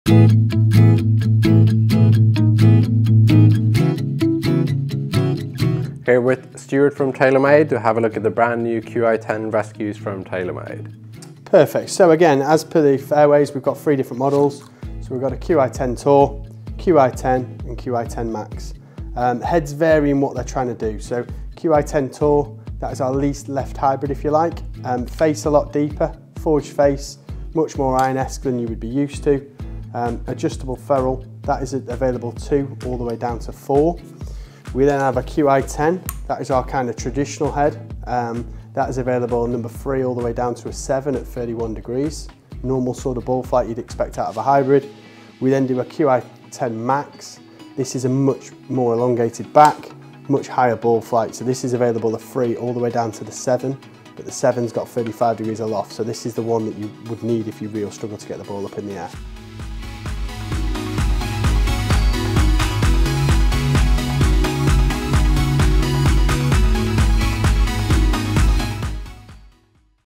PGA Pro Lars's TaylorMade Qi10 Rescue Review
JamGolf PGA Pro talks with Stuart from TaylorMade to review the release of the QI10 rescue.